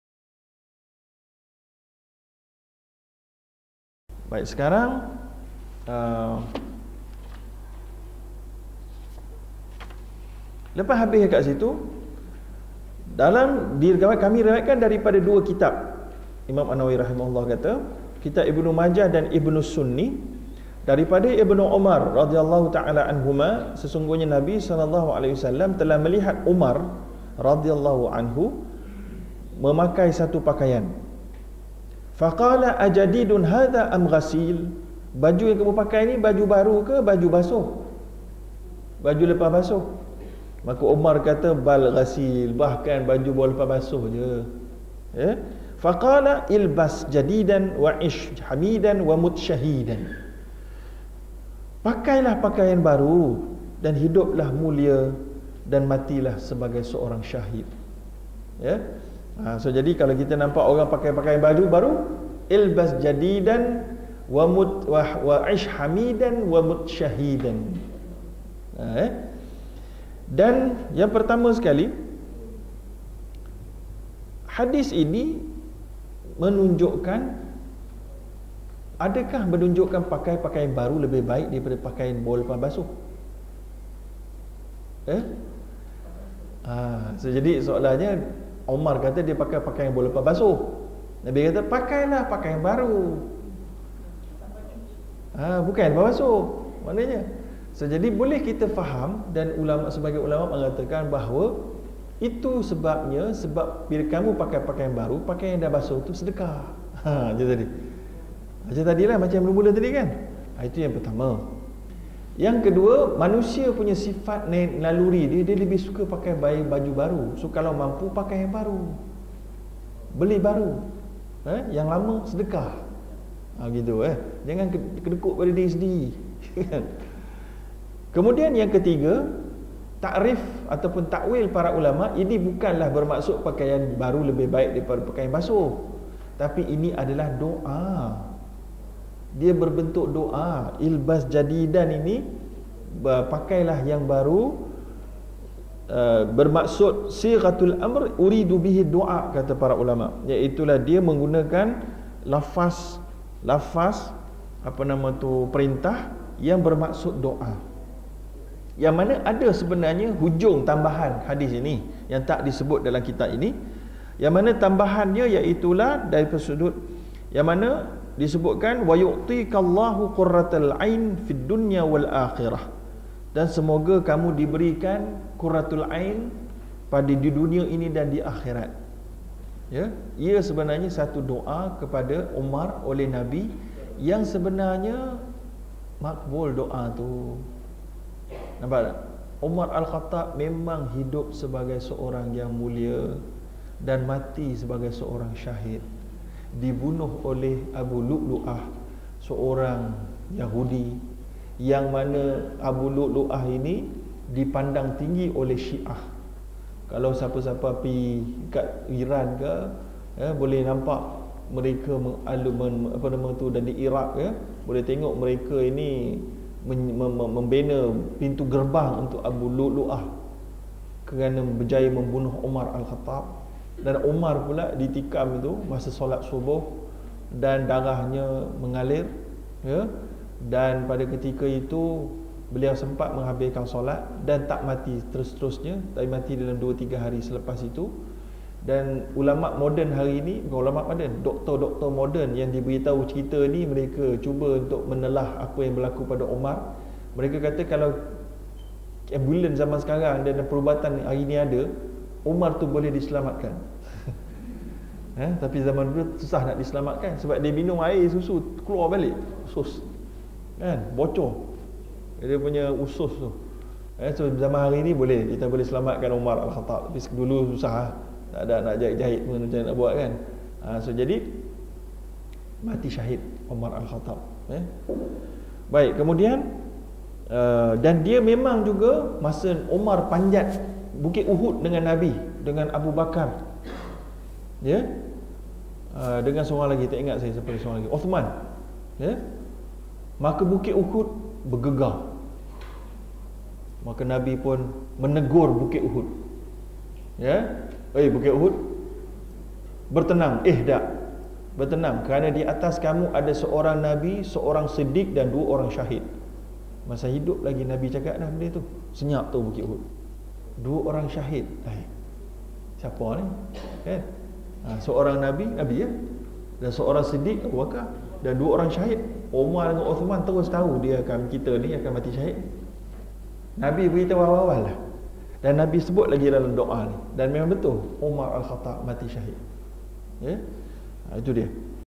Ustaz Dr. Zaharuddin : Pakaian Baru vs Basuh
Sedutan rakaman talaqqi kitab Al-Azkar siri 8